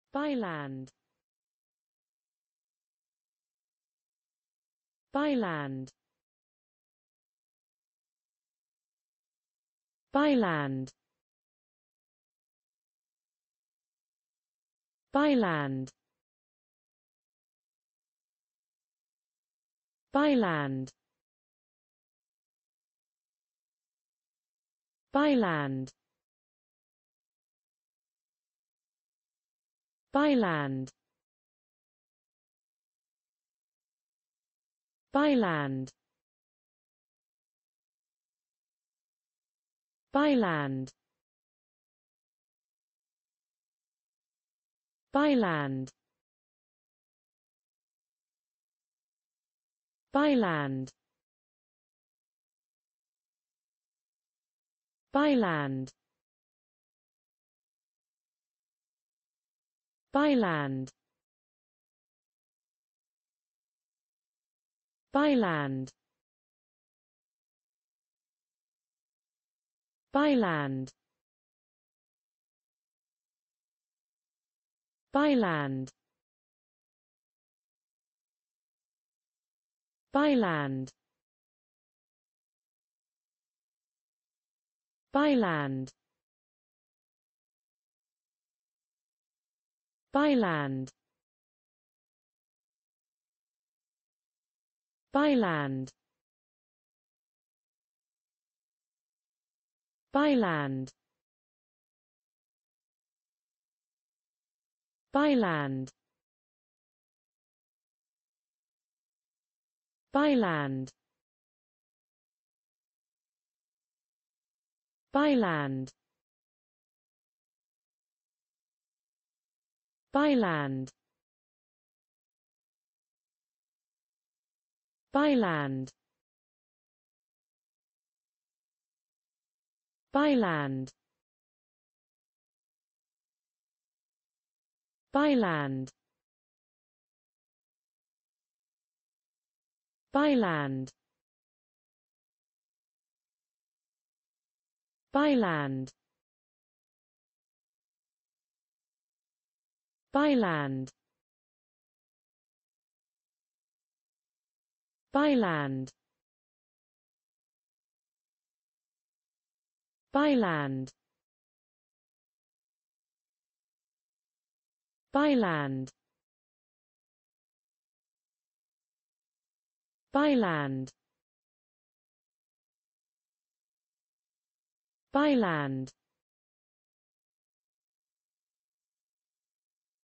by land. How to pronounce the english word by land .Start with B. Learn from me.
Follow me!Learn from me how to pronounce the english word by land.This video shows you how to pronounce the English word by land .